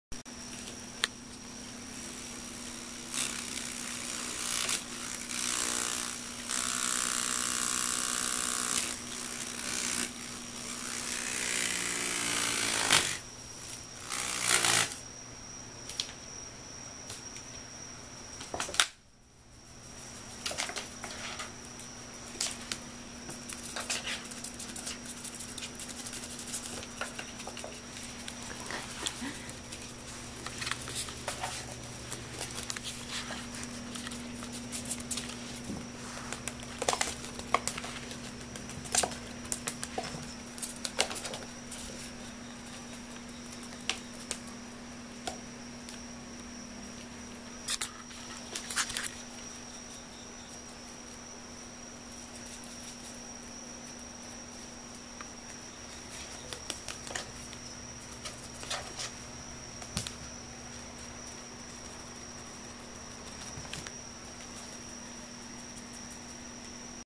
どうしても羽がほしくてしょうがない猫-Cat wants a feather stuck in the fan
扇風機の羽に鳥の羽を入れて音をだす遊びが大好き❤食いついてきます。

【TopBuzz】でも好評あめちゃん動画配信中！